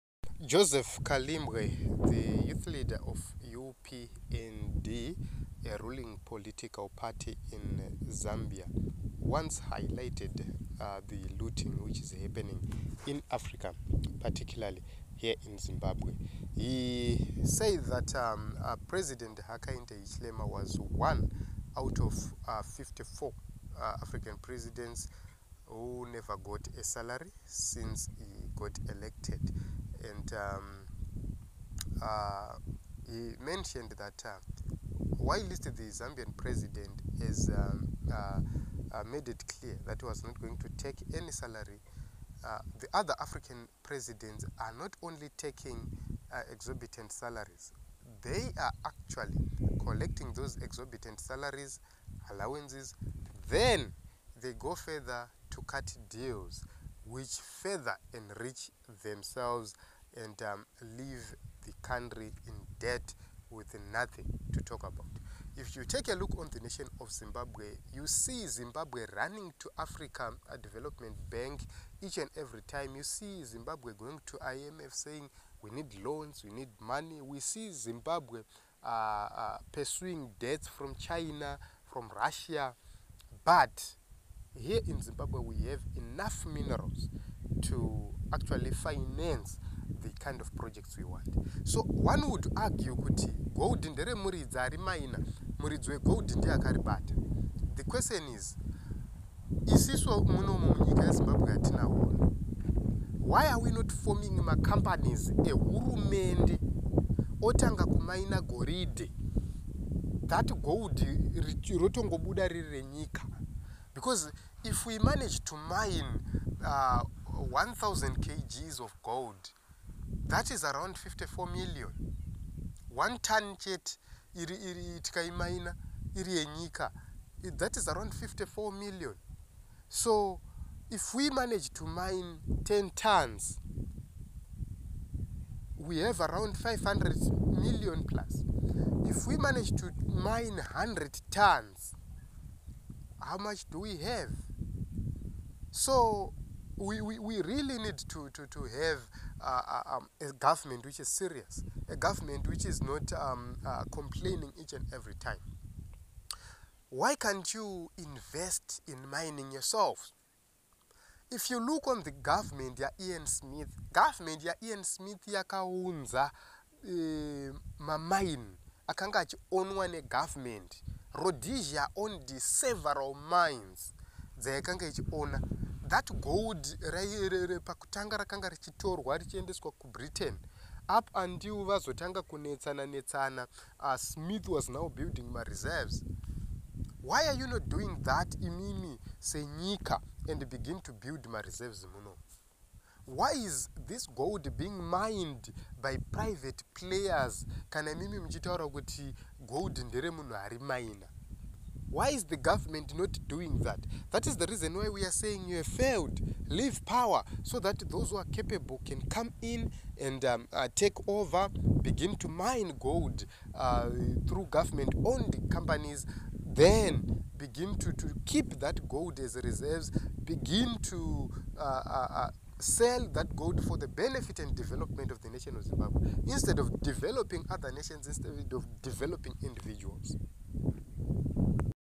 President Mnangagwa must repent and Arrest corruption masterminds
We have enough to end poverty